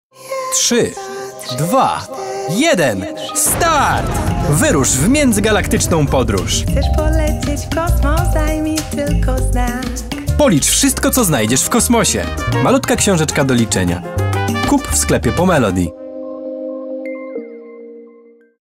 KOSMOS - malutka książeczka do liczenia 🪐 | pomelody
3️⃣...2️⃣...1️⃣... START!!! 🚀

💥 KOSMICZNA książeczka do liczenia JUŻ DOSTĘPNA!!! 💥

na początku było... nic ...

a co dalej?

1 Słońce ☀️
2 bieguny Ziemi 🌎
3 statki kosmiczne 🚀

Co jeszcze możesz znaleźć w kosmosie? SPRAWDŹ! 💫

Grube kartki (12 stron), zaokrąglone rogi i MINI rozmiar powodują, że książeczki nadają się dla dzieci od 2 roku życia. 👍

Zobacz także drugą część serii: “Woda, czyli malutka książeczka do szukania”  💧

Już niebawem do malutkich książeczek dołączy także  “Ziemia, czyli malutka książeczka do rymowania”. 🌎

W książeczce “Kosmos” znajdziesz kod QR, który odblokowuje w aplikacji pomelody piękną, kilkuminutową PODRÓŻ DŹWIĘKOWĄ, bogatą stylistycznie i barwowo. 🤩

Zadbaj o wszechstronny rozwój Twojego dziecka! ✨